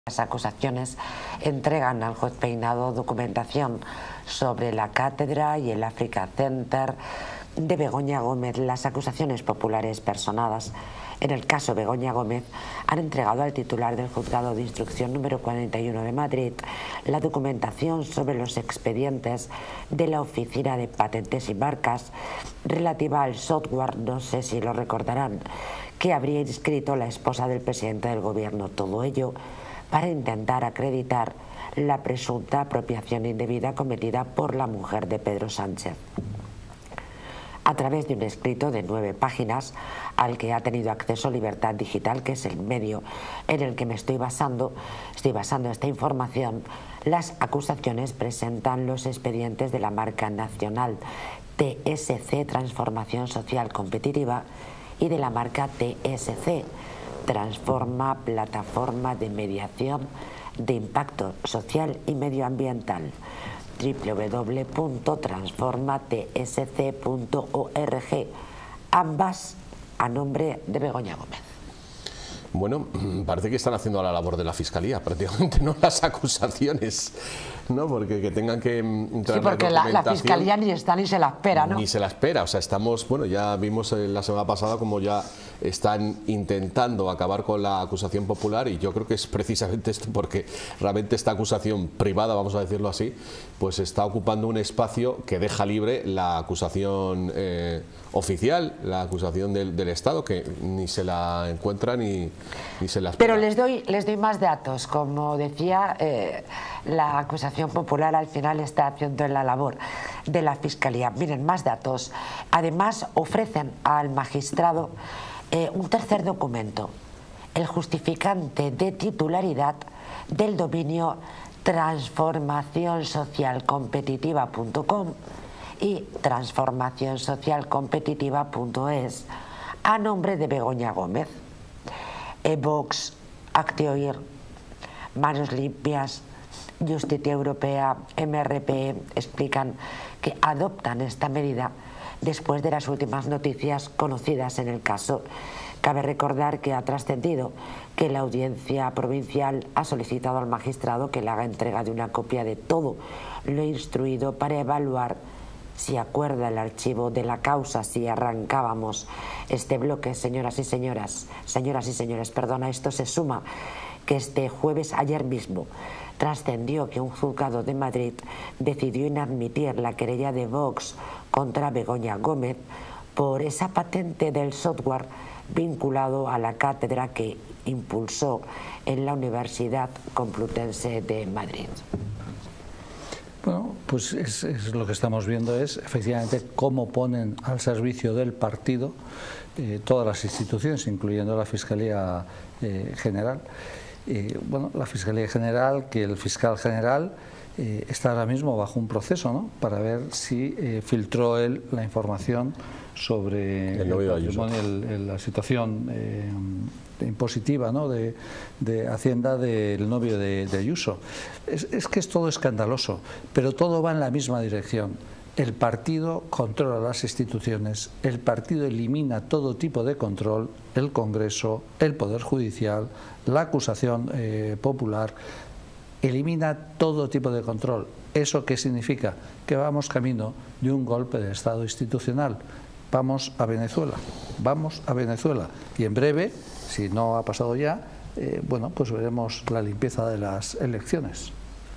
🔴ÚLTIMA HORA CASO BEGOÑA GÓMEZ🔴 NUEVAS PRUEBAS LLEGAN A PEINADO SOBRE LA CÁTEDRA Y EL ÁFRICA CENTER
En este vídeo, te traemos las últimas noticias sobre el caso de Begoña Gómez y las nuevas pruebas que han llegado a la peinado sobre la cátedra y el Africa Center. Mantente informado con Distrito TV.